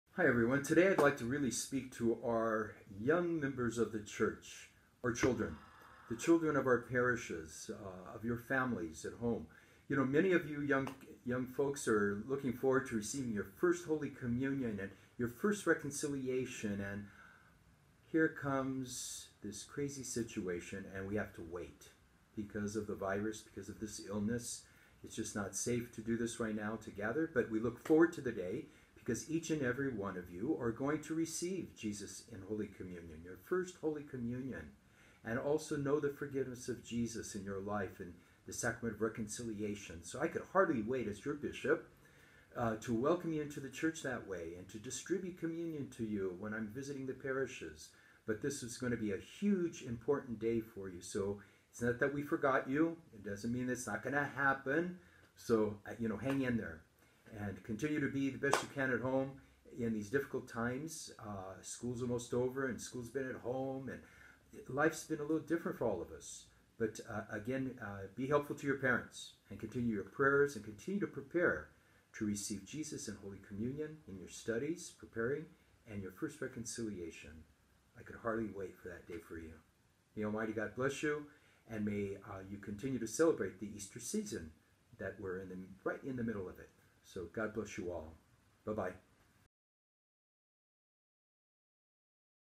Message from Bishop Cotta for Children preparing for First Communion
Bishop Cotta shares some words of encouragement for those who are looking forward to receiving their First Communion.